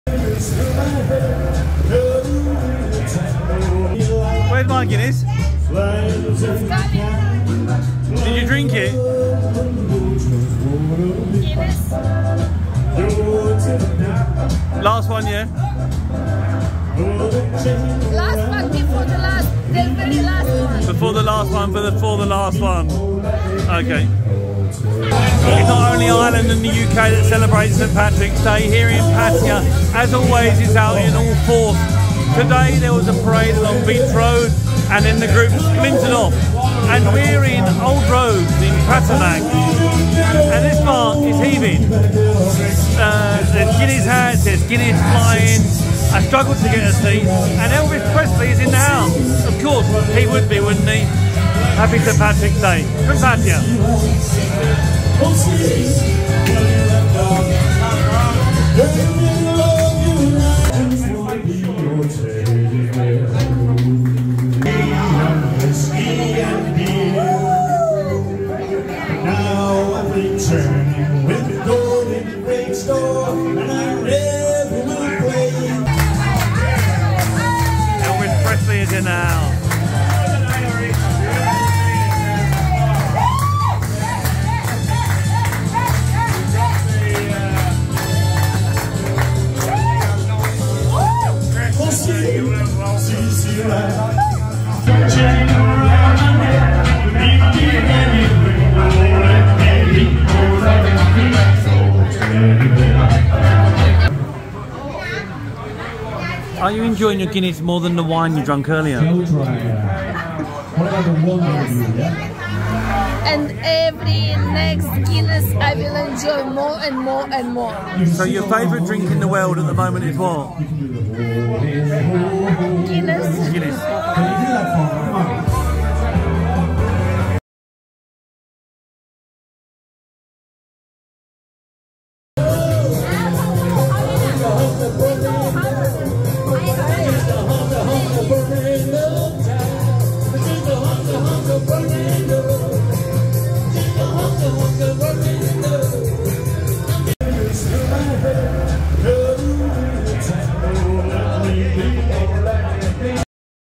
My Paddy's Day in Pattaya 2023
Pattaya does not need an excuse to get the beers flowing and so I decided to join in the fun for this year's St Patricks Day.